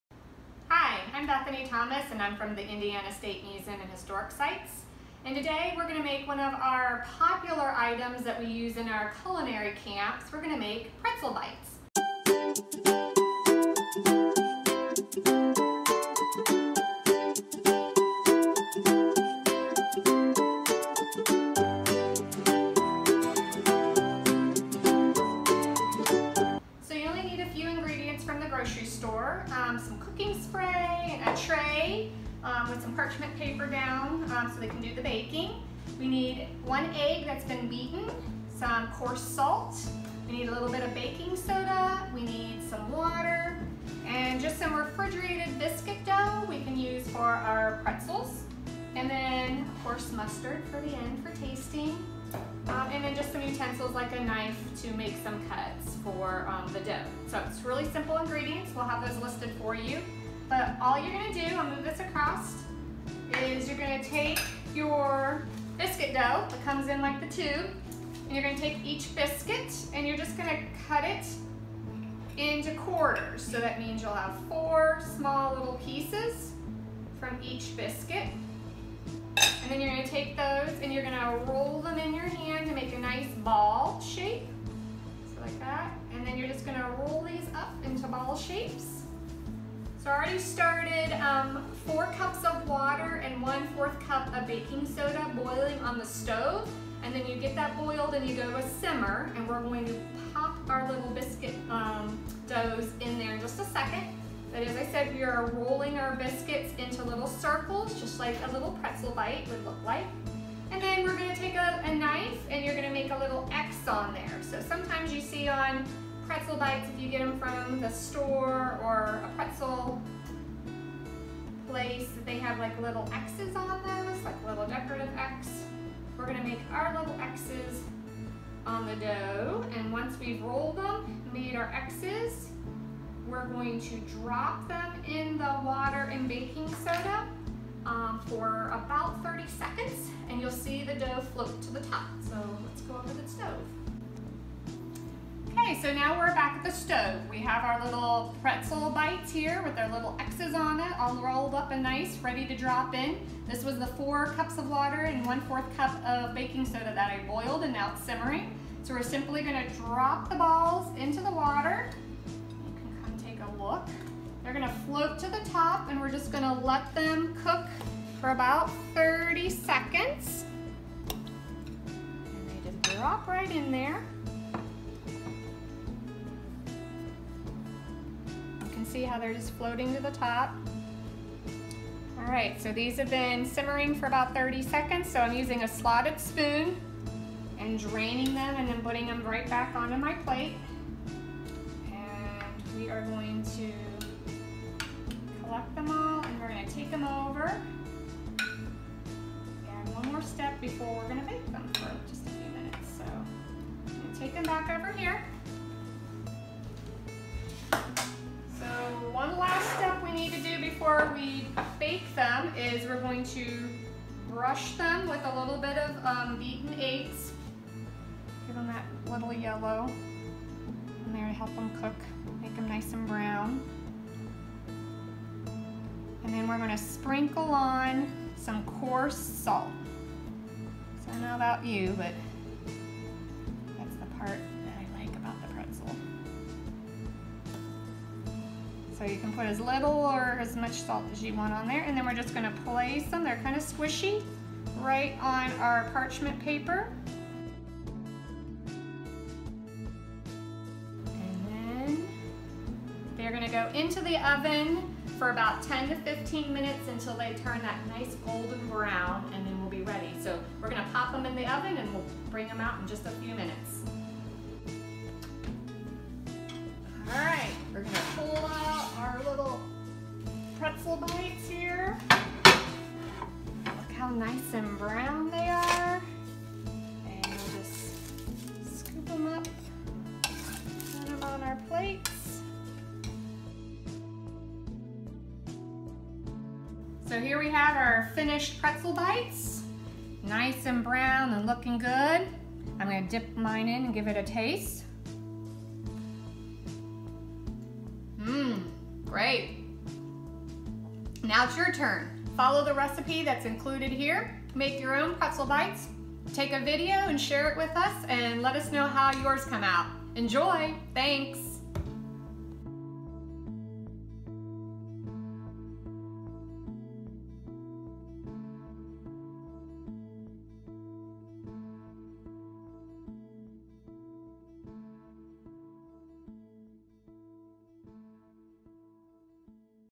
Kids Cooking: Pretzel Bites
In this video, Bethany from the Indiana State Museum shares her favorite recipe they use during Kids Cooking Camp.